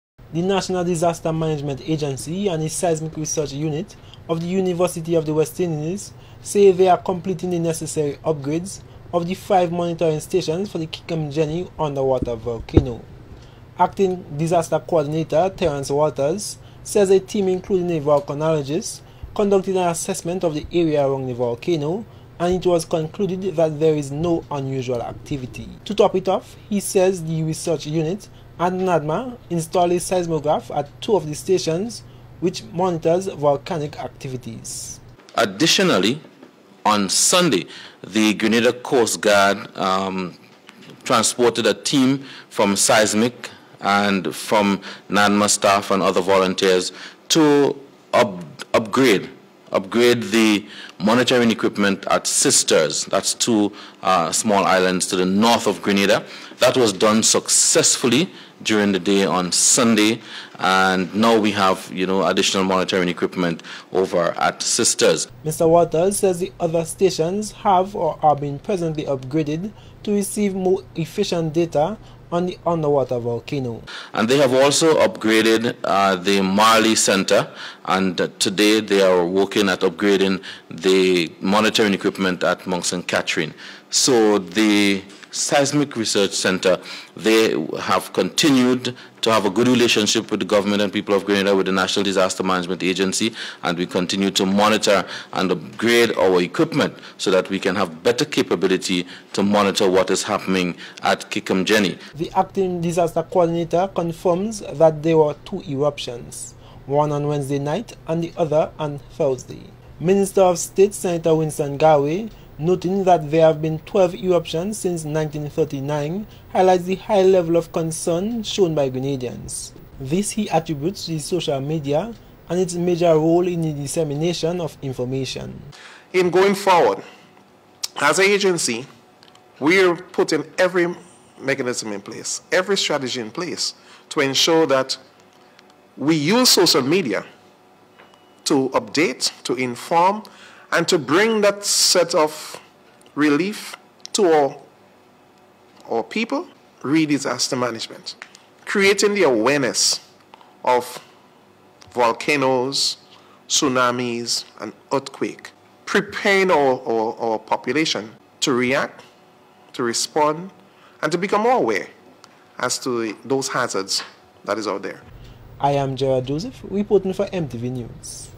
Kick em Jenny Volcano in the Caribbean
The National Disaster Management Agency and by extension Grenada is improving its ability to closely monitor activities at the Kick Em Jenny underwater volcano.
A series of seismic systems upgrades are being undertaken, at monitoring stations at Marli St. Patrick’s and Carriacou.
Gerard Joseph has more!